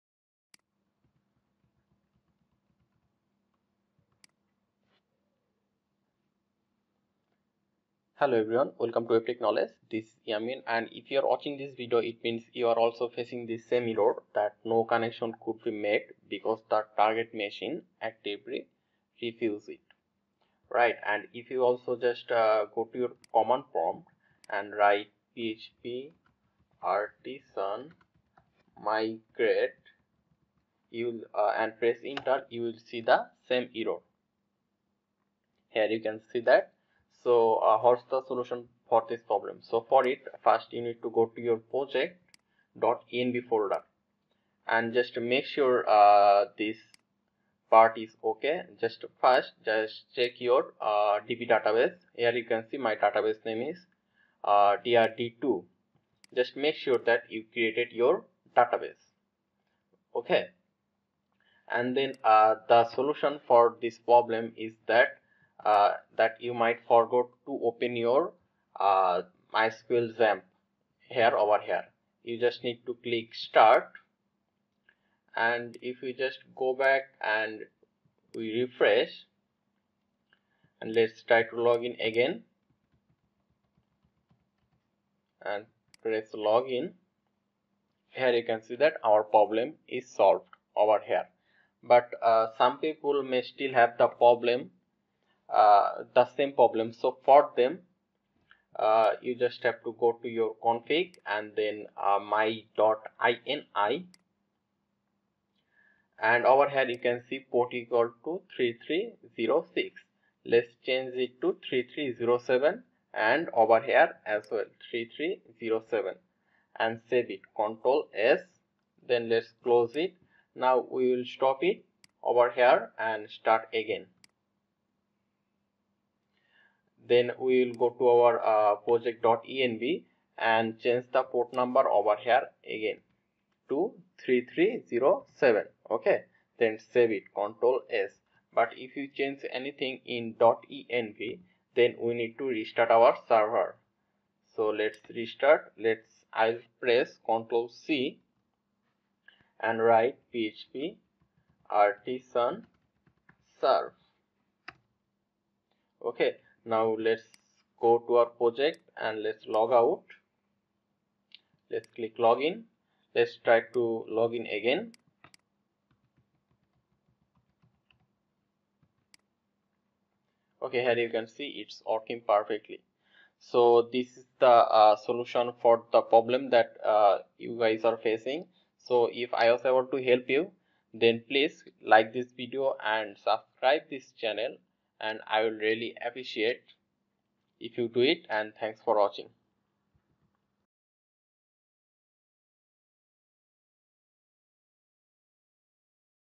How to fix no connection could be made because target machine actively refused it
1000000% SOLVED .. no connection could be made because target machine actively refused it.